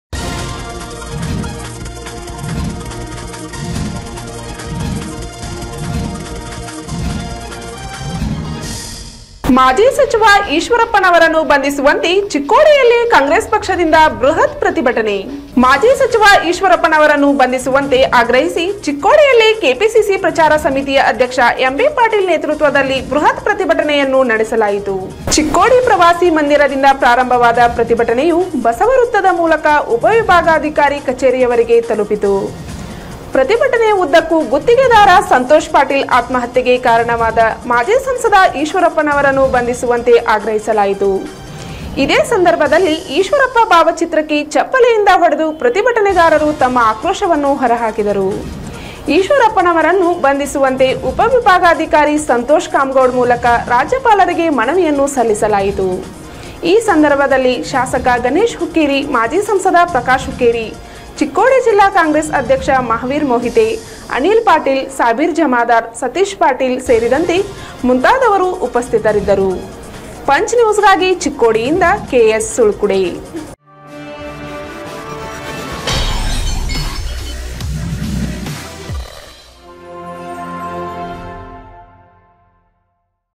ಮಾಜಿ ಸಚಿವ ಈಶ್ವರಪ್ಪ ಅವರನ್ನು ಬಂಧಿಸುವAತೆ ಚಿಕ್ಕೋಡಿಯಲ್ಲಿ ಕಾಂಗ್ರೆಸ್ ಪಕ್ಷದಿಂದ ಬೃಹತ್ ಪ್ರತಿಭಟನೆ.
ಮಾಜಿ ಸಚಿವ ಈಶ್ವರಪ್ಪನವನ್ನ ಬಂಧಿಸುವAತೆ ಆಗ್ರಹಿಸಿ ಚಿಕ್ಕೋಡಿಯಲ್ಲಿ ಕೆಪಿಸಿಸಿ ಪ್ರಚಾರ ಸಮಿತಿಯ ಅಧ್ಯಕ್ಷ ಎಂ.ಬಿ.ಪಾಟೀಲ ನೇತೃತ್ವದಲ್ಲಿ ಬೃಹತ್ ಪ್ರತಿಭಟನೆಯನ್ನು ನಡೆಸಲಾಯಿತು.